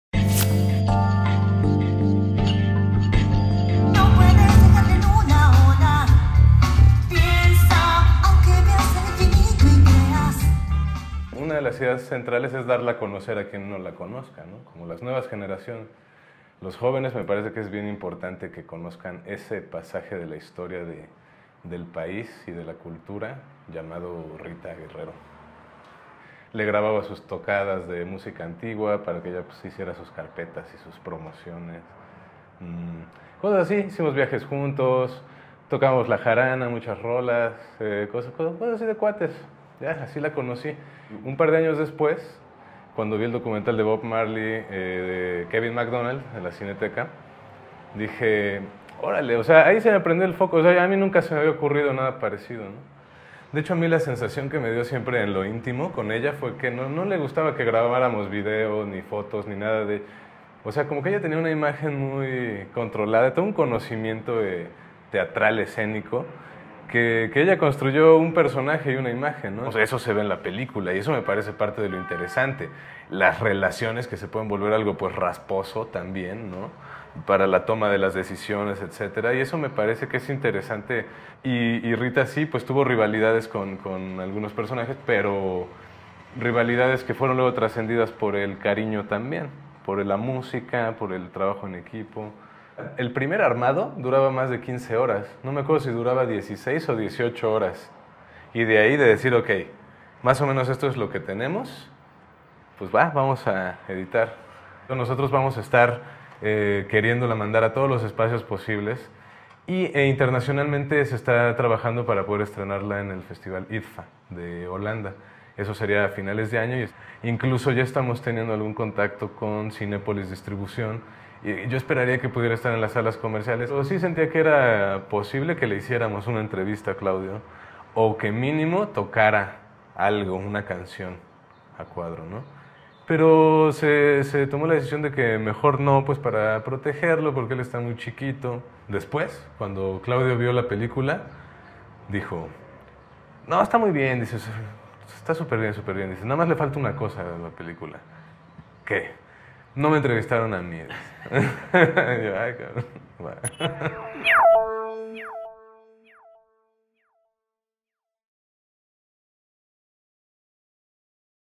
Una vida llena de talento | Reporte Indigo 1493
Arturo Díaz Santana logró con “Rita, el documental”, hablar de la figura central entorno a la agrupación Santa Sabina, abarcando su aspecto personal pero sobretodo su dedicación excepcional a la música, al teatro y al performance